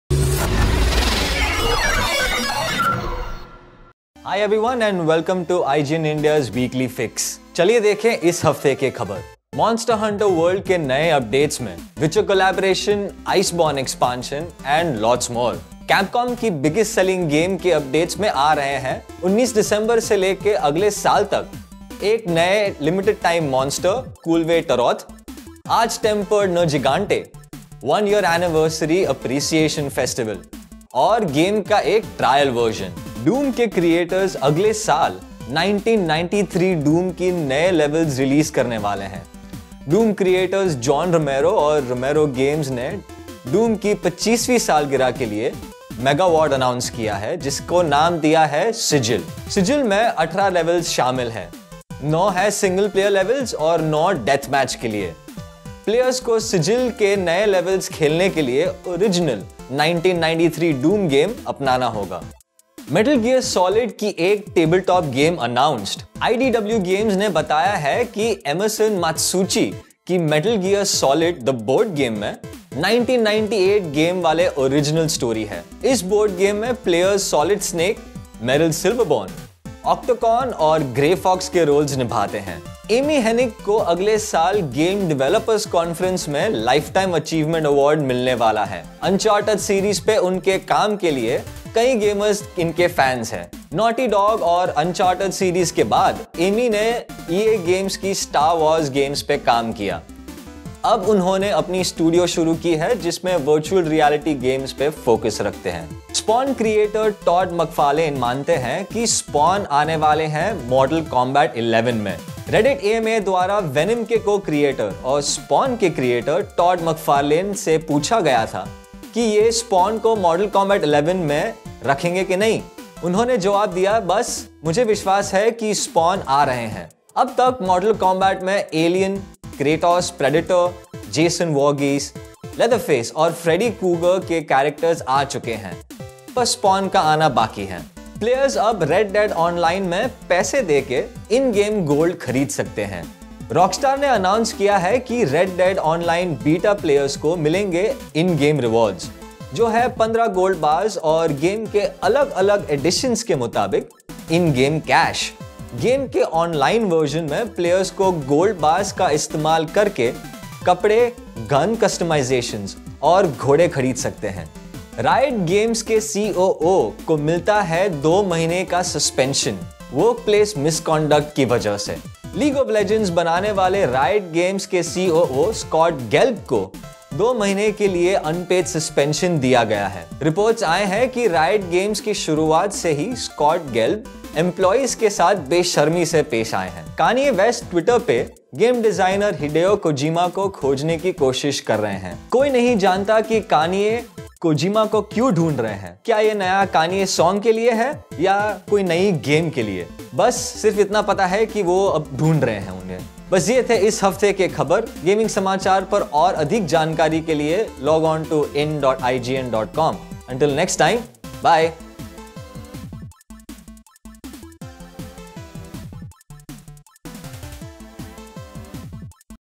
IGN Hindi Weekly Fix 5
Iss hafte, Kanye dhoondte hai Kojima ko, Geralt aaye Monster Hunter: World mein aur MGS ab ek table-top game hai!